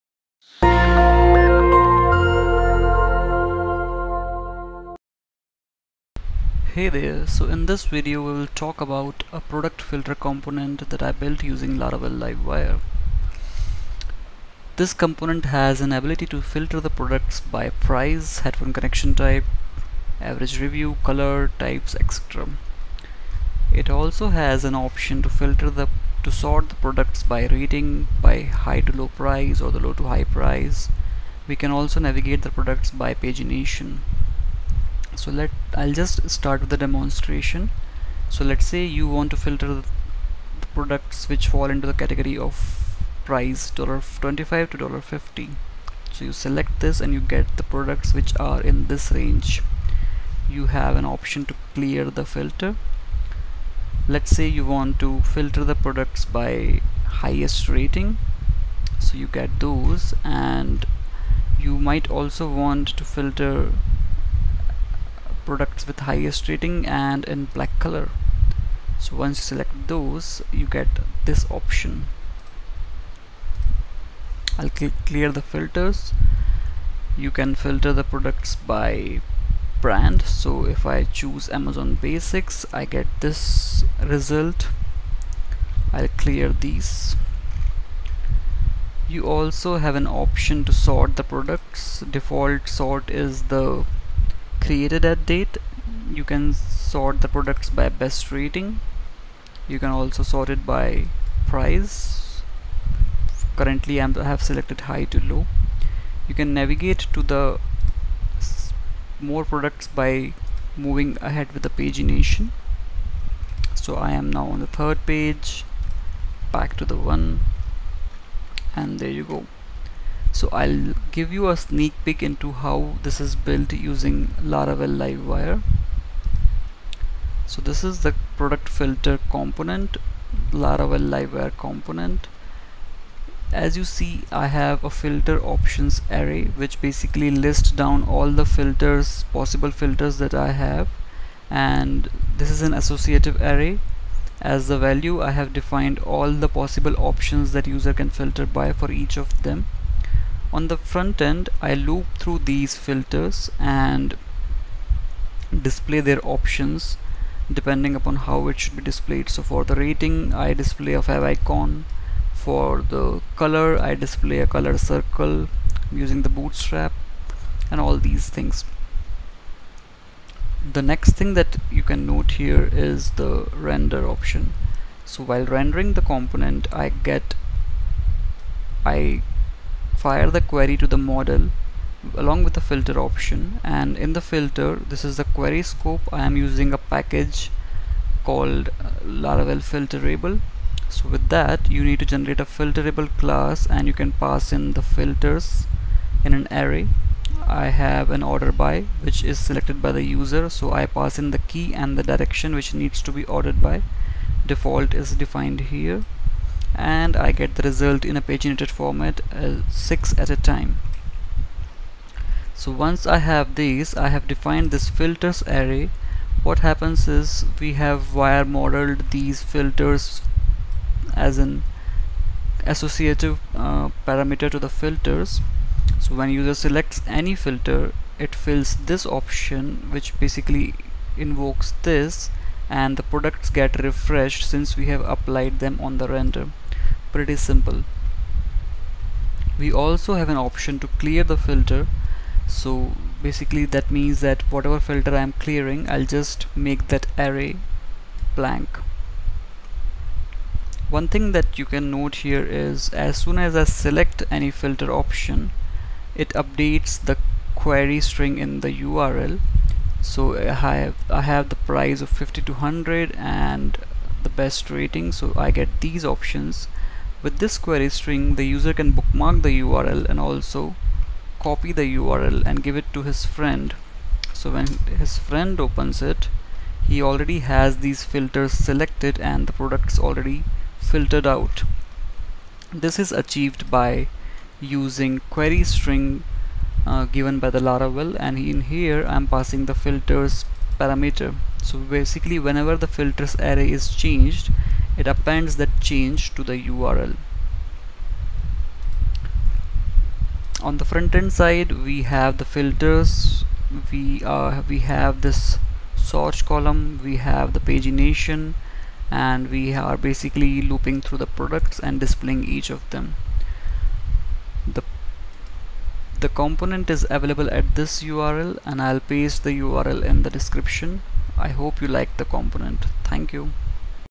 Product Filter Component using Laravel Livewire
Demonstration of a product filter component build using Laravel Livewire. Filter the products by price, rating, colour, brand etc. Ability to sort and paginate as well.